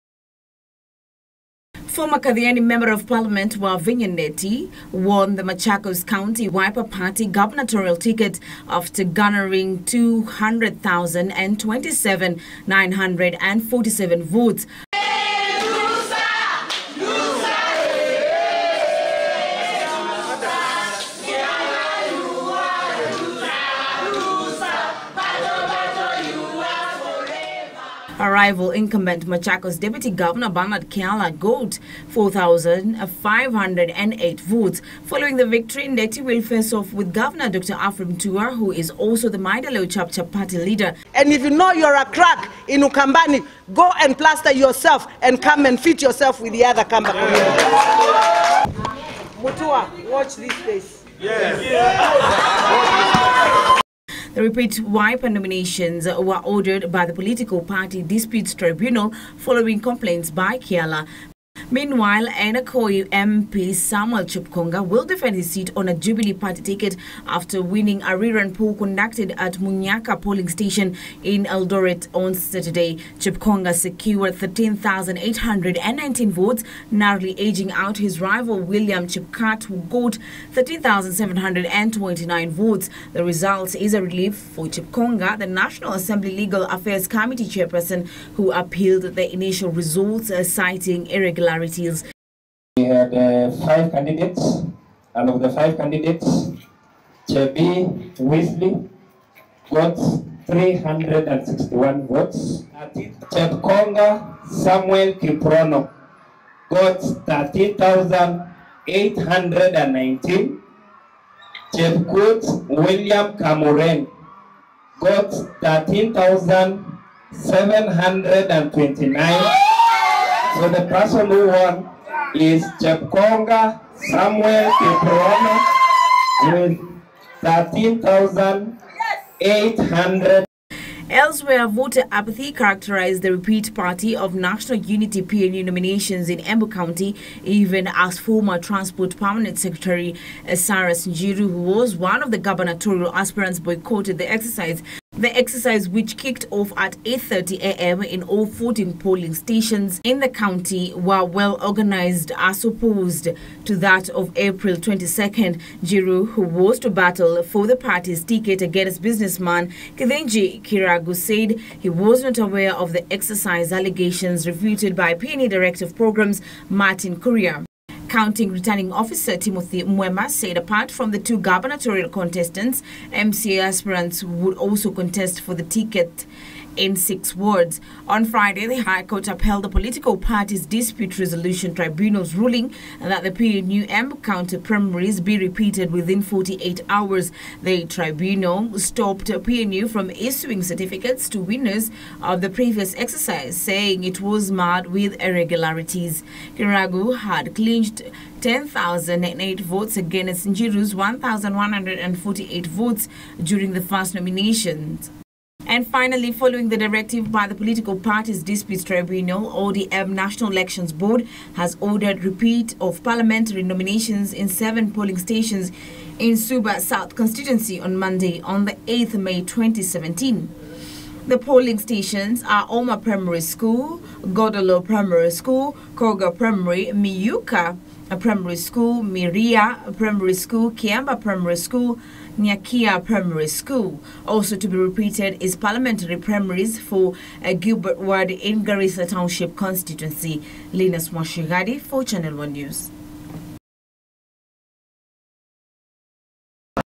Wavinya Ndeti clinches Wiper gubernatorial ticket
Former Kathiani lawmaker Wavinya Ndeti is set to face off with Governor Dr Alfred Mutua in the Machakos County gubernatorial contest come August 8th after securing the Wiper Democratic party ticket. Ndeti floored Deputy Governor Bernard Kiala by more than 223, 000 votes in the repeat party primaries conducted on Saturday.  Meanwhile  ODM parliamentary nominations in 7 polling stations in Suba South constituency will be repeated on Monday on the 8th may 2017following orders from the Political Parties’ Disputes Tribunal.